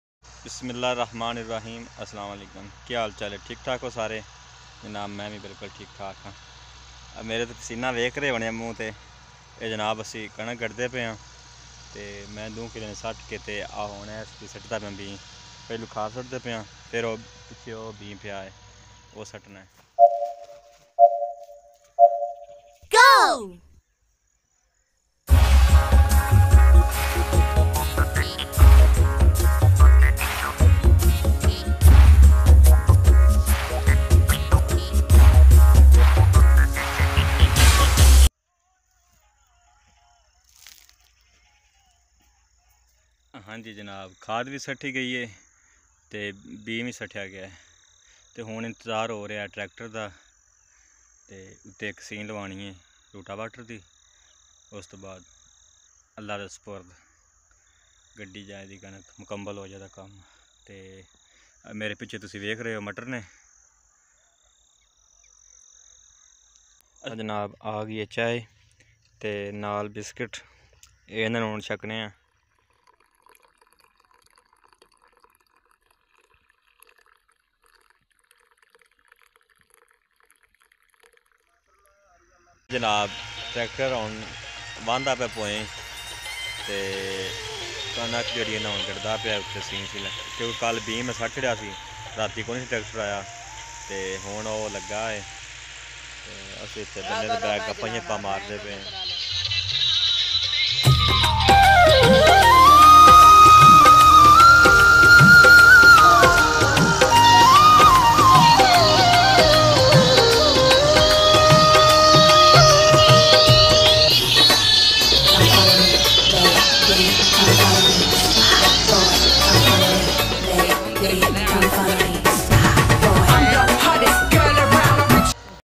Instead of wheat in Pakistan || گندم کی بجائی || Gandum Ki Kasht Krty Howy
I am playing wheat today. I am very happy today
Gandum ki kasht